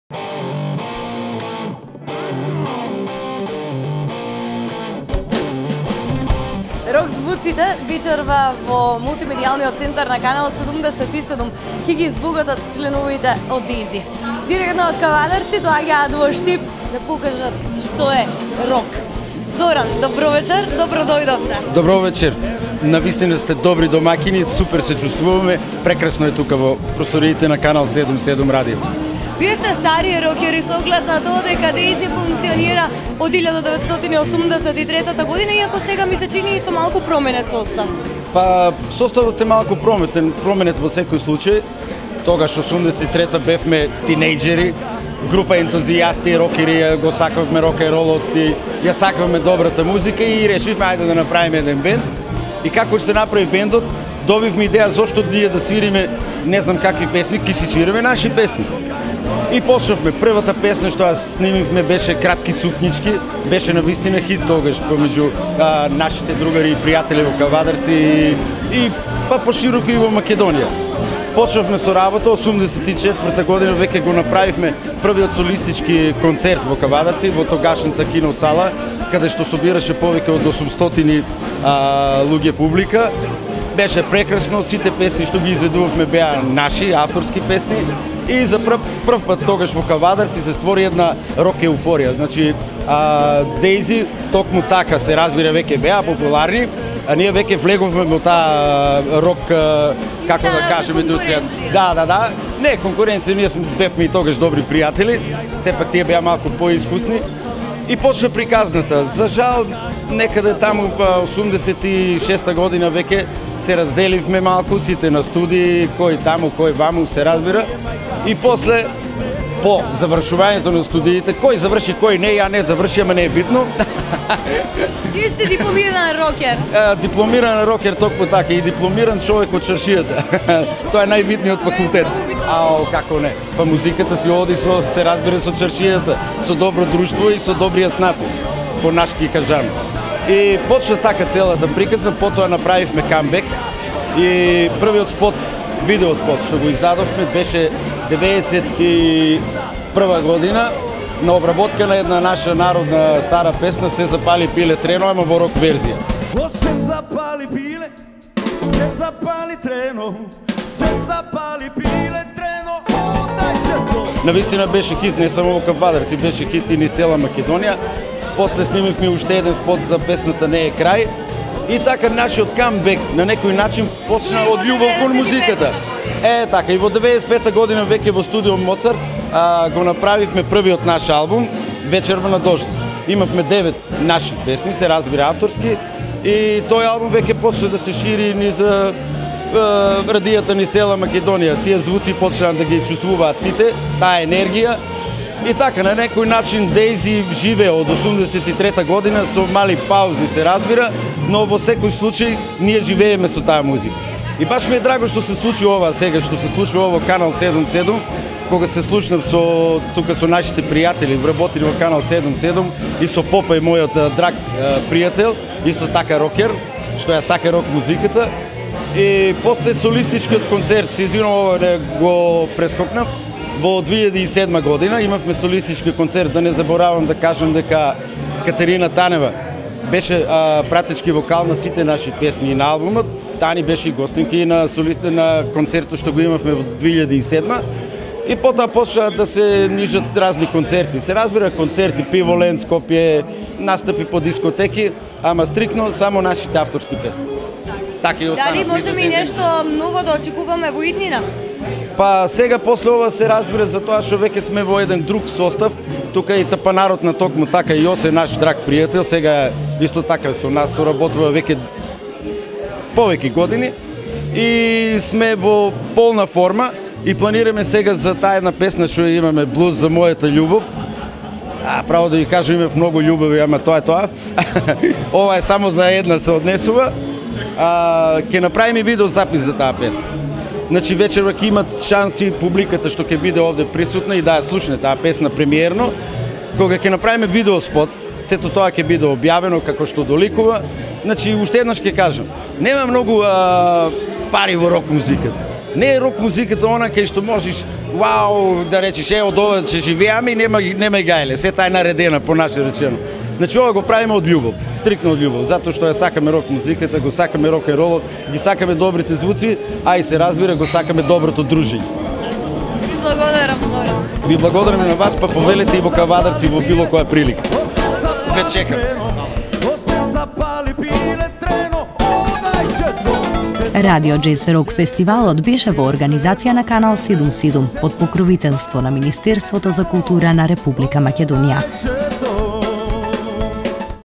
Дејзи - Џез Рок Фестивал
Разговор со Зоран Станоев - пејач во Дејзи